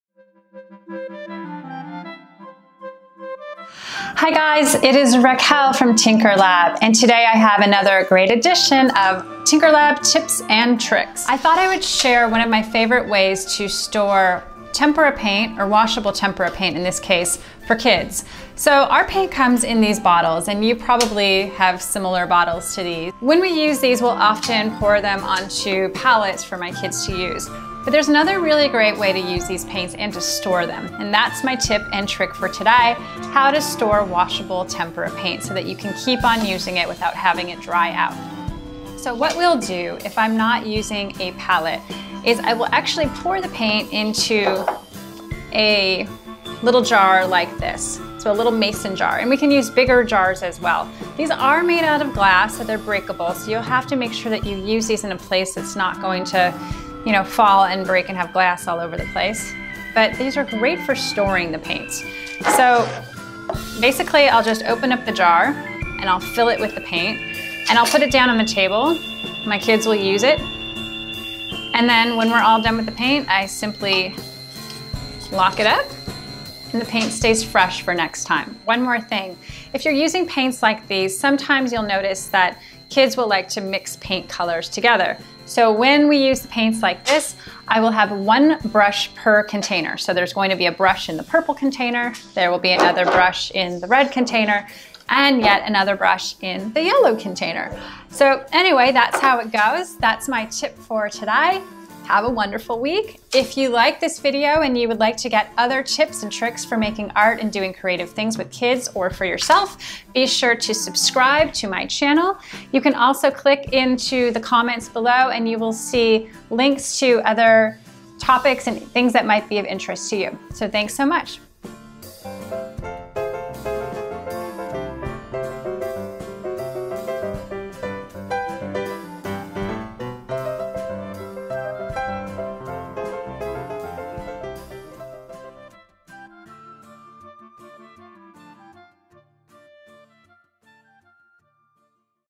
Art Tips and Tricks: How to Store Tempera Paint so that it won't dry out.
Mini Mason Jars
Favorite Fat Brushes for Preschool Kids
Favorite Tempera Paints